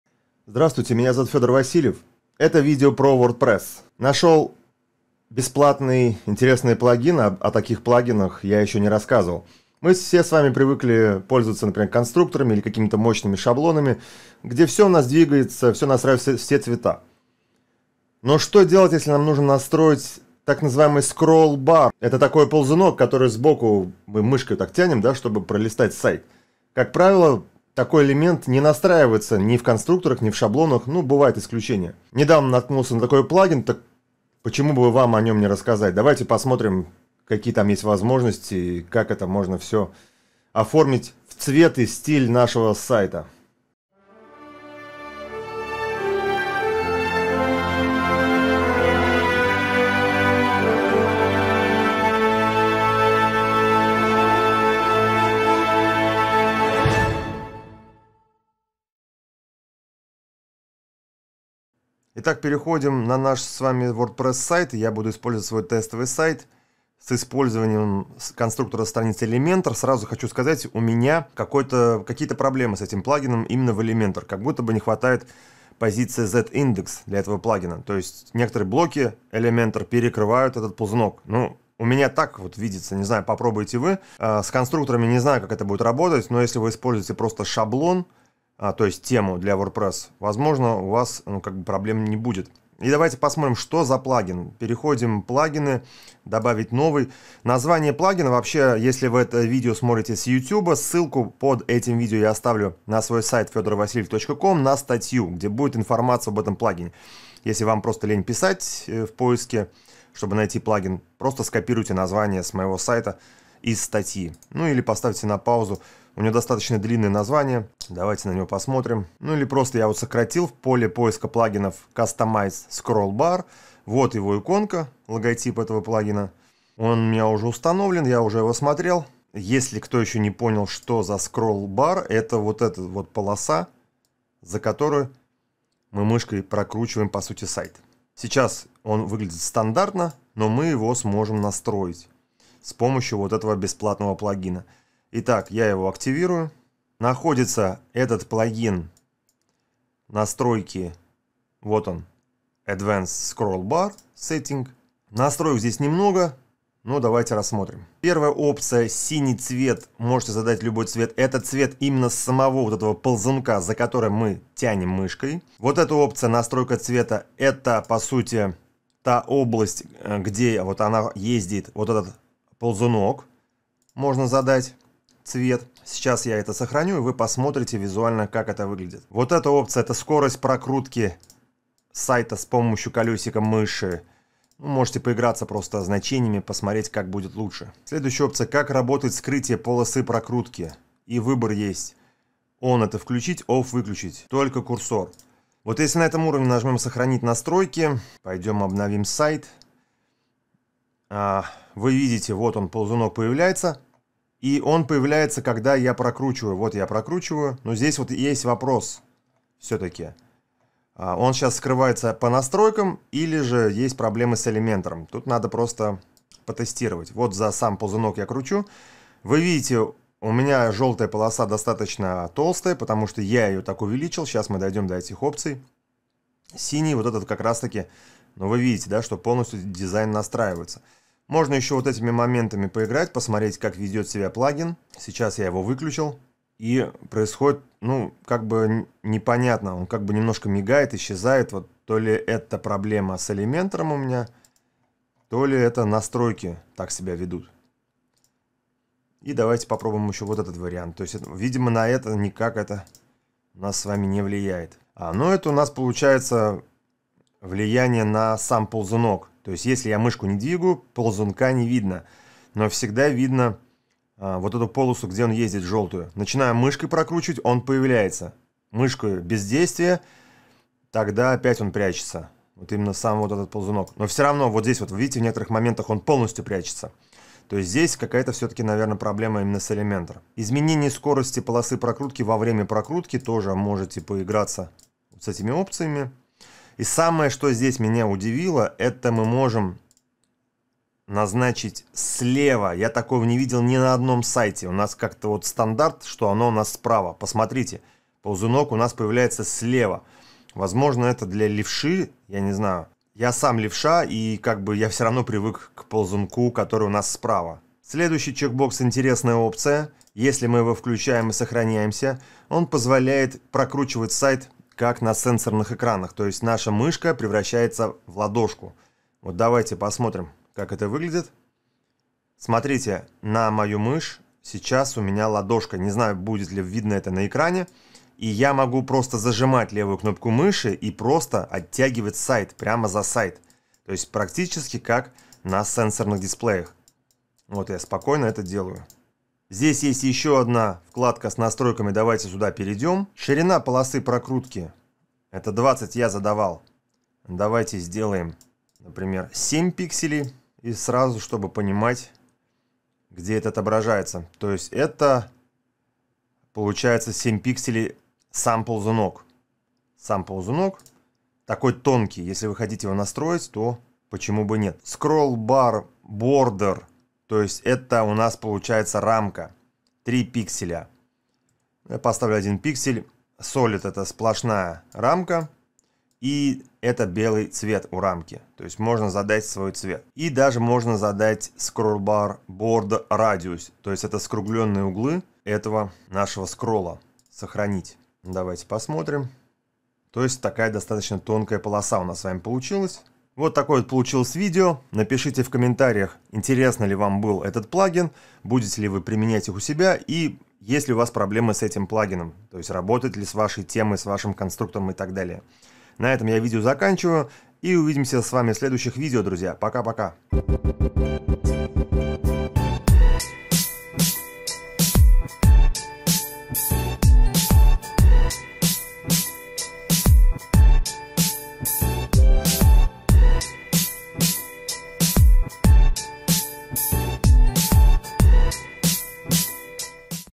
Настройте полосу прокрутки (scrollbar) на вашем WordPress сайте
В этом видео я покажу бесплатный плагин для WordPress, с помощью которого вы сможете оформить свой дизайн полосе прокрутки (scrollbar) браузера вашего сайта, без знания кода.